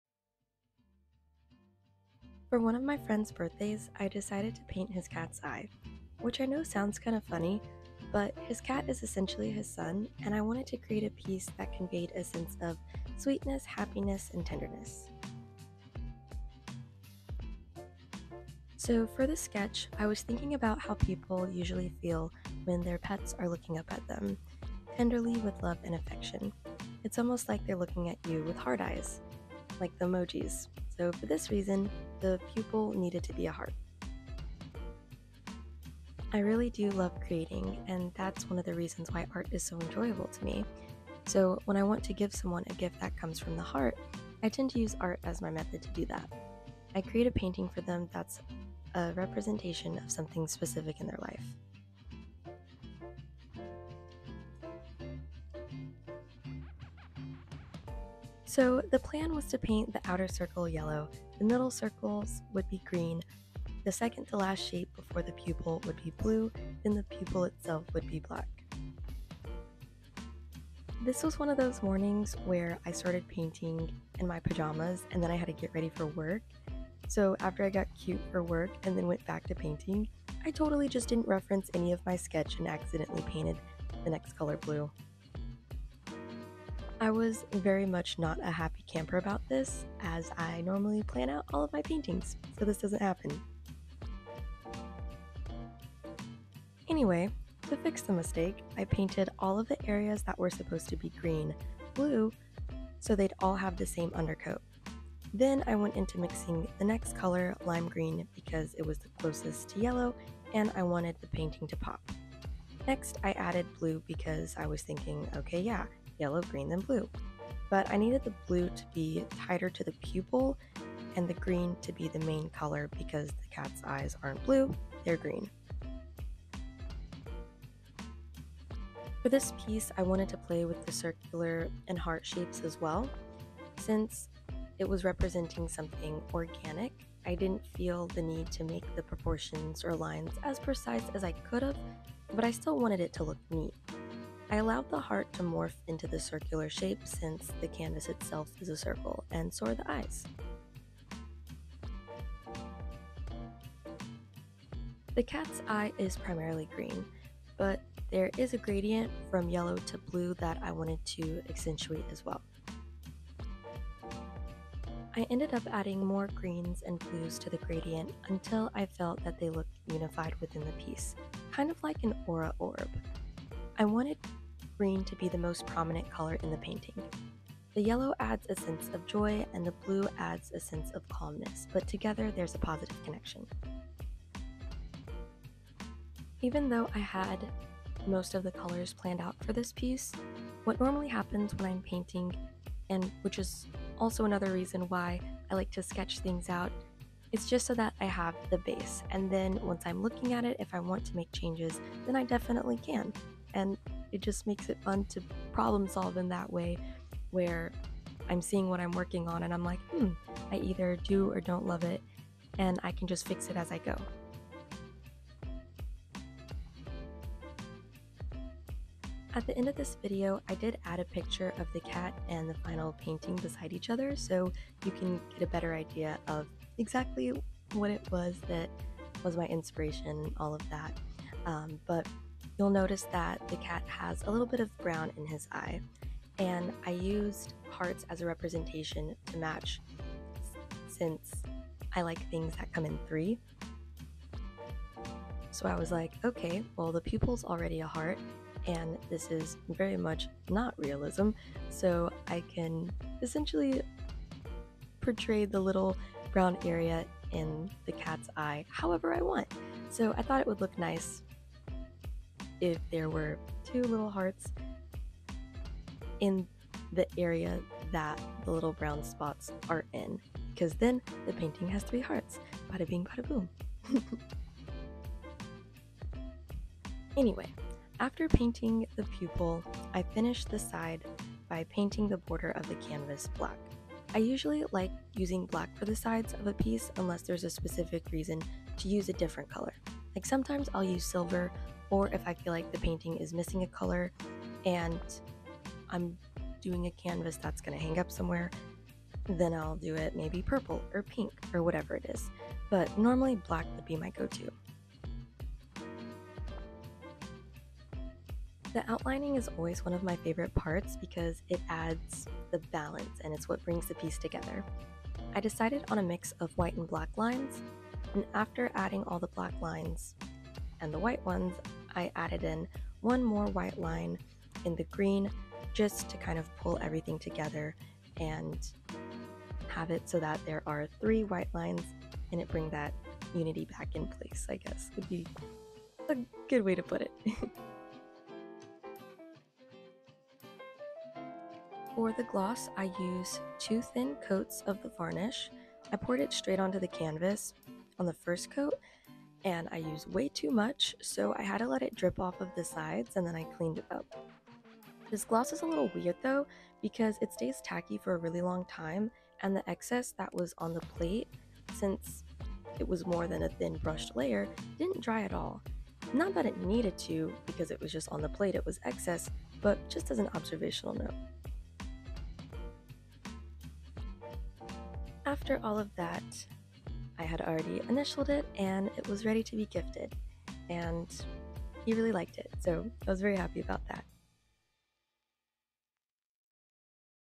Draw With Me - Acrylic Painting Process 🎨 Canvas Painting ⏳ Art Vlog
Hello Everyone and Welcome To Chelsea Elizabeth Channel.

Join my art process today as I craft a heartfelt birthday gift for my friend—an artistic representation of his beloved cat's eye. I want to share the inspiration behind the painting, aiming to evoke feelings of sweetness, happiness, and tenderness that our furry friends bring into our lives.

Let's start from initial sketches to an unexpected painting mishap that leads to a unique problem-solving adventure. The carefully chosen color palette—yellow, green, and blue—that brings vibrancy to the cat's eye, with the pupil crafted in the shape of a heart, symbolizing the deep connection we share with our pets.

As I add the finishing touches, including a black border for balance and a glossy varnish for a polished look, witness the transformation of the canvas into a meaningful work of art.  🎨🐾